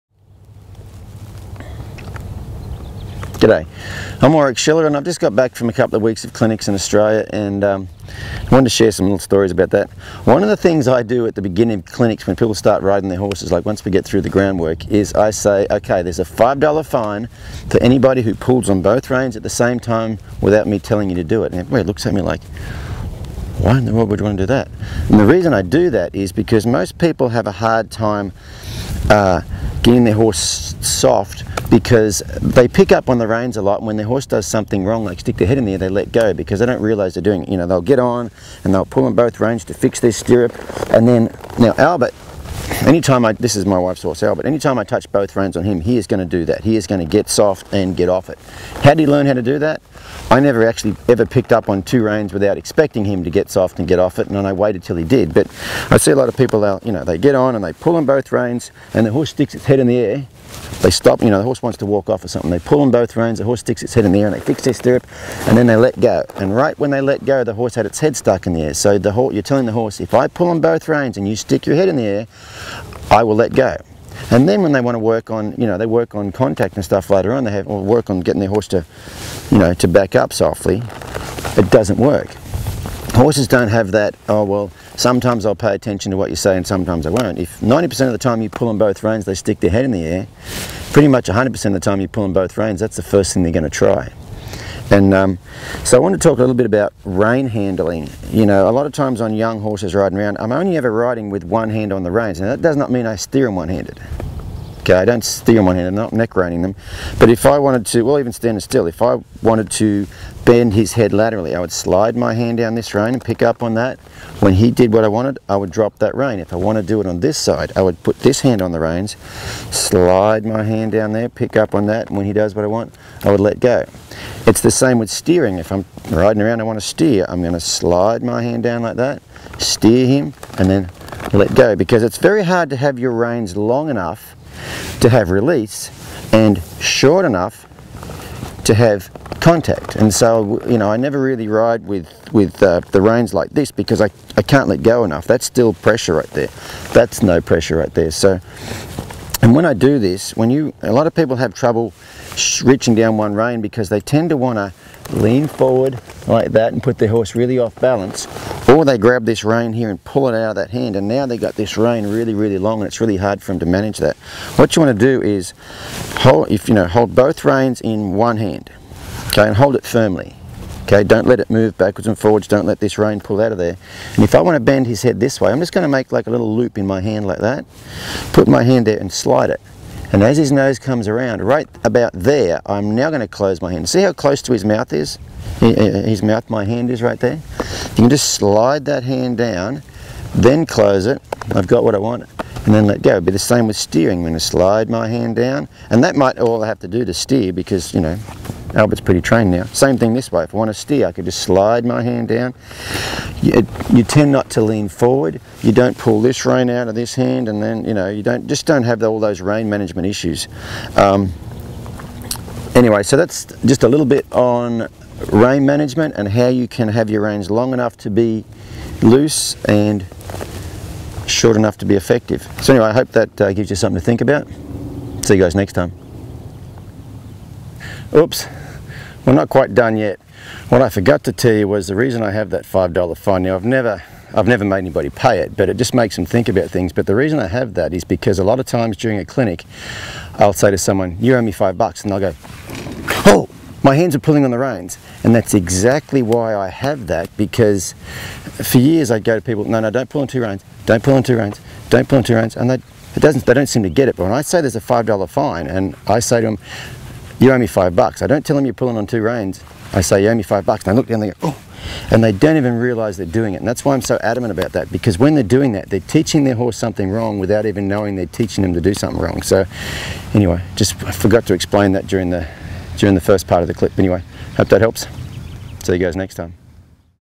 Blog #7 - Rein management
A video blog on riding with two reins.

We have hundreds of Full Length Training Videos filmed with real horses with real problems in real time available